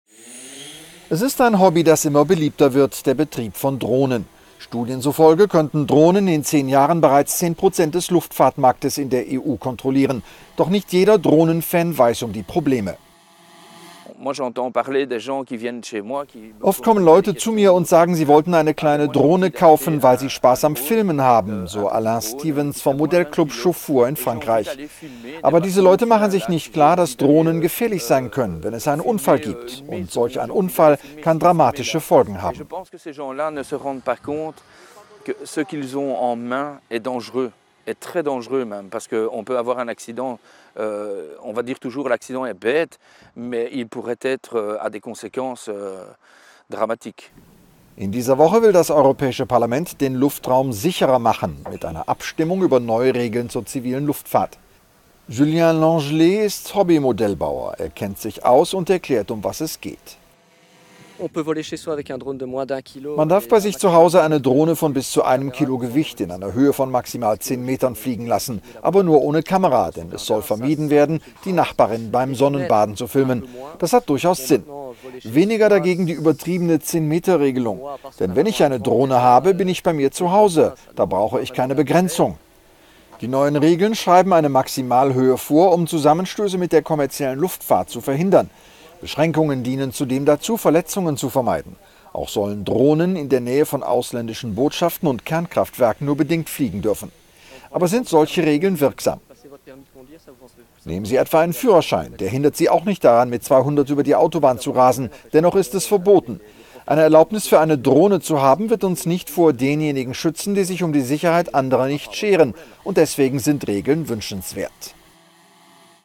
EU-Parlament berät neue Regeln zu Dronen
"Augen im Himmel" werden immer beliebter - und die Probleme immer mehr…